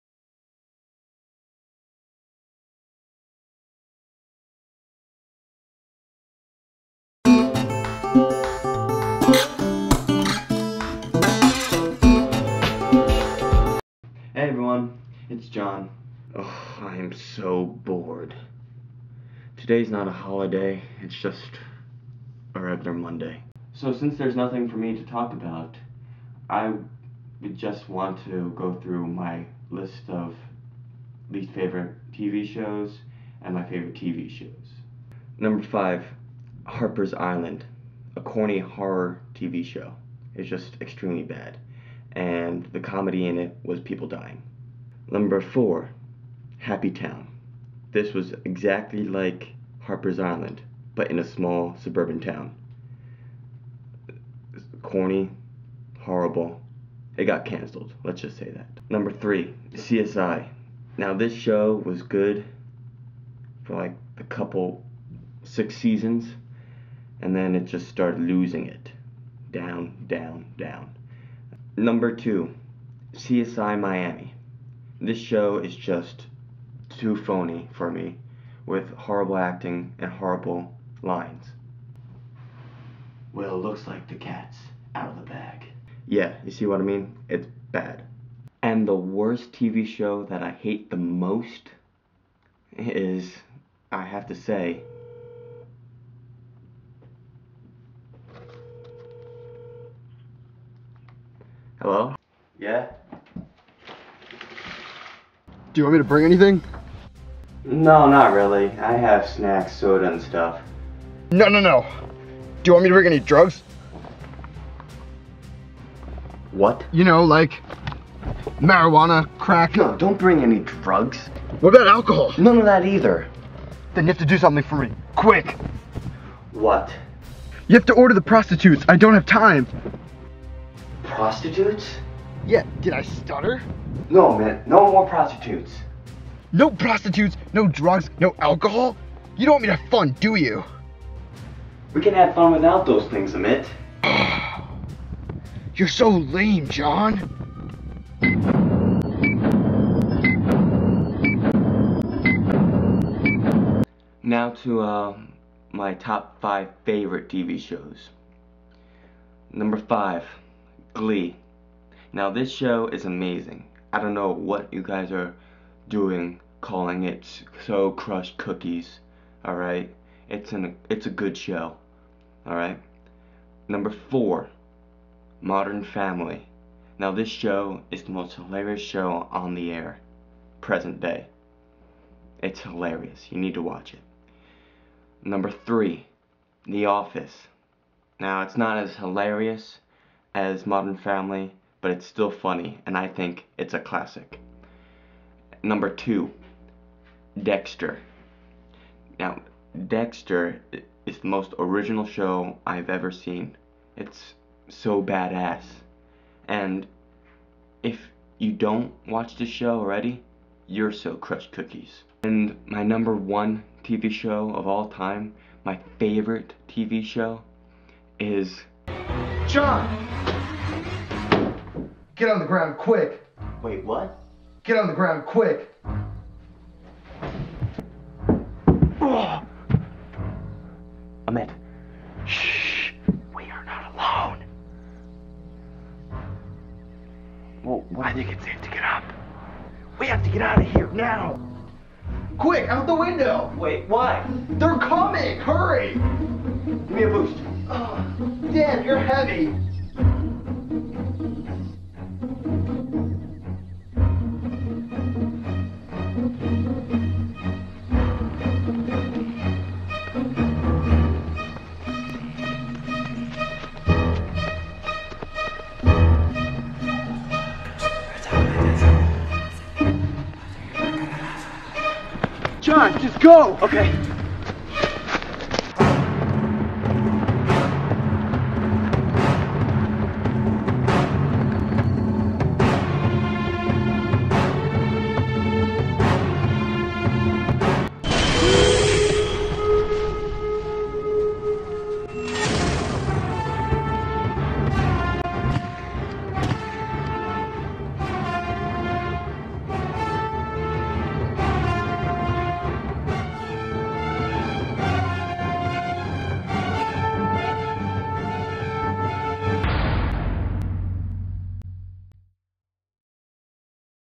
John's video blog 3
My 3rd video Blog. Going Through my least favorite and favorite tv shows